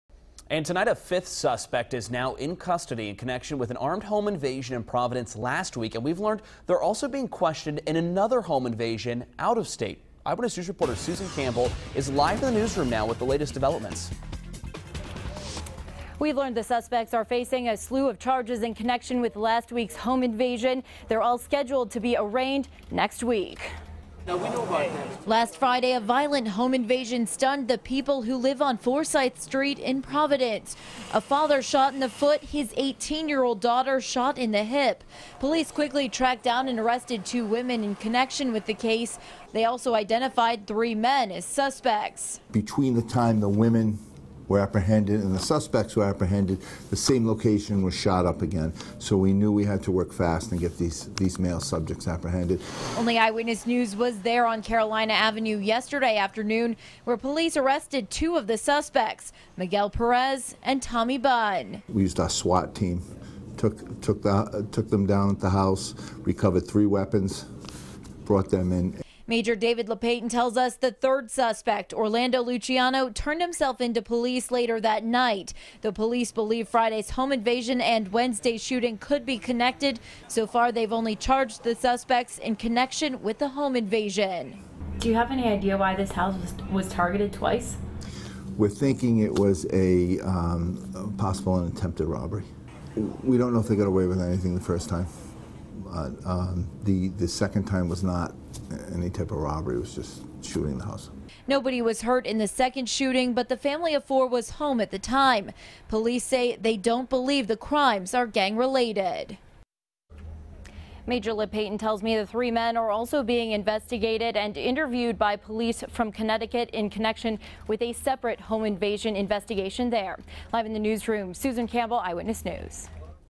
Fifth suspect in violent home invasion surrenders to police
Orlando Luciano, 22, turned himself in as a suspect in last Friday's violent home invasion on Forsyth Street in Providence.